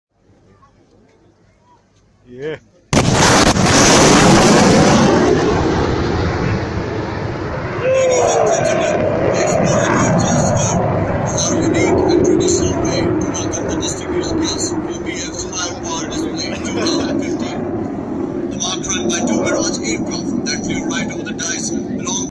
Sonic Boom | PAF Mirage 3 from 'Bandits' Squadron breaks sound barrier.
Pakistan Air Force Mirage 3 from 7th "Bandits" Squadron break the sound barrier over Sonmiani.